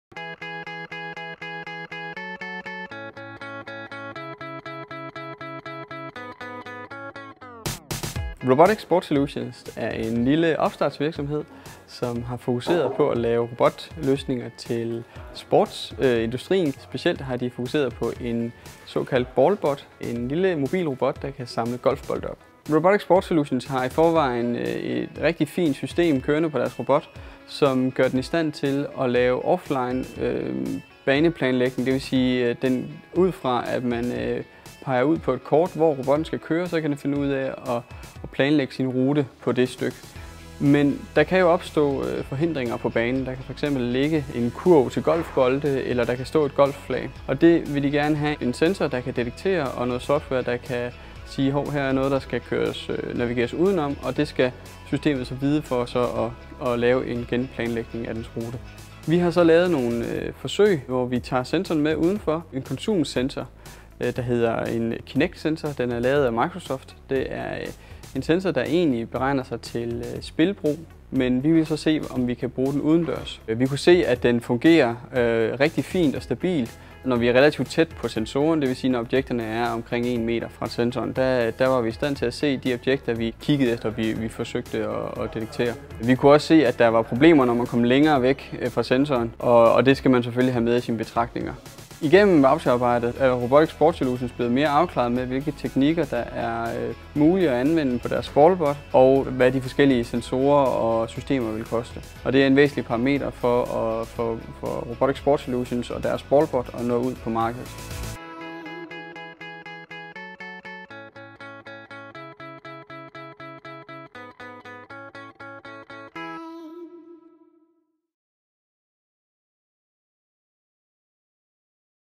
ROBOTT-NET: Cost Effective Sensors for Outdoor Navigation for Robotic Sport Solutions
Robotic Sport Solutions, RSS, develops autonomous mobile robots for maintenance purposes in sports. Navigation both indoor and outdoor in a dynamic environment is a challenging task especially when costs have to be kept to a minimum.

Through the ROBOTT-NET voucher work, Robotic Sport Solutions has been equipped with a clear vision of the various techniques available to apply on their BallBot.